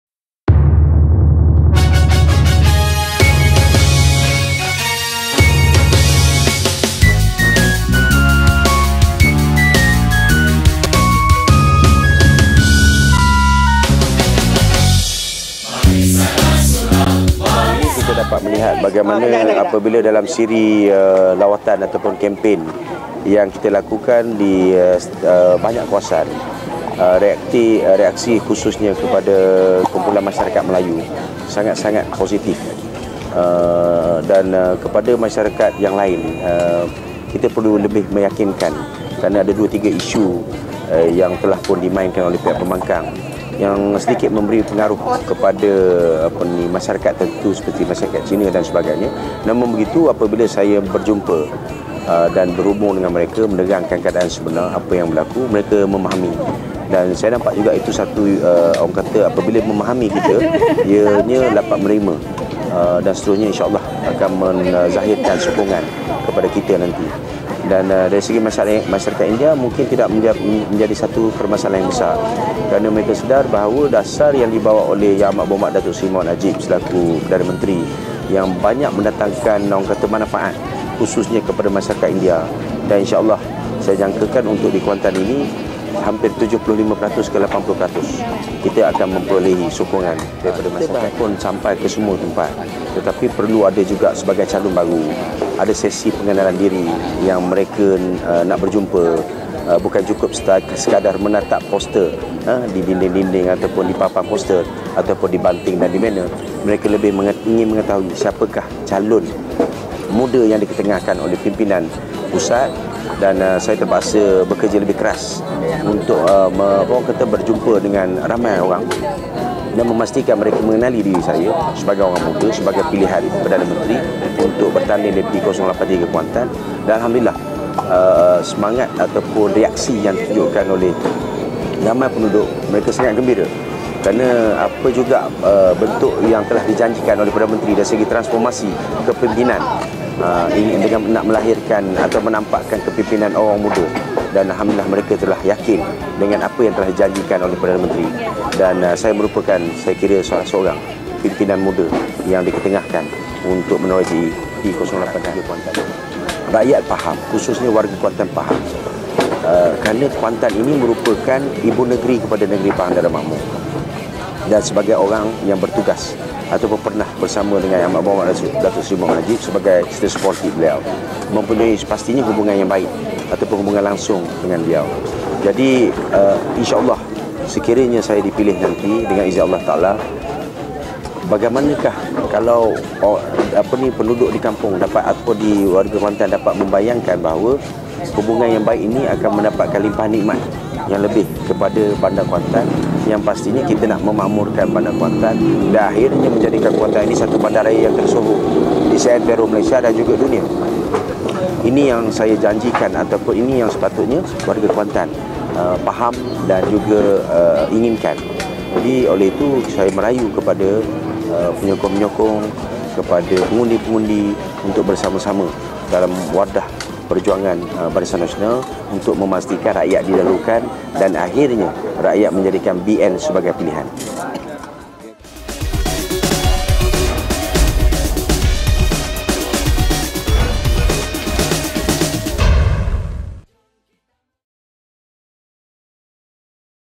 PRU 13 : Janji Saya Jadikan Kuantan Bandaraya Tersohor Di Dalam Dan Luar Negara - Suffian Awang
KUANTAN, 2 Mei -- Calon Barisan Nasional (BN) Parlimen Kuantan, Datuk Suffian Awang berkata, berdasarkan siri-siri lawatan kempen yang beliau lakukan di seluruh kawasan Parlimen Kuantan, respon serta keyakinan rakyat, khususnya orang Melayu terhadap BN kini semakin pulih dan meningkat.

Bagaimanapun menurut beliau, ada segelintir lagi pengundi, khususnya dari kaum Cina perlu lebih diyakinkan memandang pembangkang telah memainkan beberapa isu yang sedikit sebanyak telah mempengaruhi sejumlah minoriti pengundi dari kaum tersebut.

"Namun apabila saya bertemu dengan mereka dan menerangkan apa cerita sebenar tentang isu-isu yang dimainkan oleh pihak pembangkang, nampaknya mereka mula memahami apa cerita sebenar di sebalik isu-isu tersebut.

"Apabila kita menjelaskan isu-isu tersebut kepada mereka, nampaknya mereka sudah mula mahu menerima dan memahaminya, dan insyaallah saya yakin, kefahaman mereka tentang isu tersebut akan mereka terjemahkan, atau zahirkan dengan memberikan undi kepada BN pada pilihanraya pada 5 Mei nanti," jelas beliau.

Manakala dari respon masyarakat India di sekitar Parlimen tersebut, Suffian berkeyakinan bahawa sokongan dari kaum tersebut sudah mula kembali meningkat terhadap BN dan tidak seperti sebelum ini.

"Daripada pertemuan dan perbualan saya dengan kaum India, mereka sedar dan nampak bahawa dasar transformasi kerajaan yang diperkenalkan oleh Datuk Seri Najib, banyak memberi peluang dan manfaat kepada masyarakat India.

"Berdasarkan pertemuan saya itu in shaa Allah, daripada respon positif yang diperlihatkan oleh masyarakat India di sini, saya yakin BN di Parlimen Kuantan akan memperolehi lebih 75 peratus daripada mereka,"ujarnya.

Suffian turut berjanji, sekiranya beliau terpilih sebagai Ahli Parlimen Kuantan, maka beliau akan dengan sepenuh daya dan upaya akan memberikan khidmat yang terbaik buat warga penduduk di Kuantan selaras dengan program transformasi yang sedang dilaksanakan oleh kerajaan di bawah pimpinan Datuk Seri Najib

"Matlamat saya adalah untuk membangunkan dan memakmurkan bandar Kuantan agar menjadi satu bandaraya yang tersohor bukan hanya di dalam negara ini saja, tetapi untuk dikenali sehingga ke peringkat antarabangsa.

"Ini adalah janji saya dan justeru itu, saya merayu kepada pengundi di seluruh Parlimen Kuantan, agar mereka memilih dan memenangkan BN bukan saja di peringkat Parlimen, tetapi juga diperingkat DUN agar segala perancangan untuk membangunkan Kuantan sebagai sebuah bandaraya tersohor dapat kita realisasikan," ujarnya. -MediaUMNO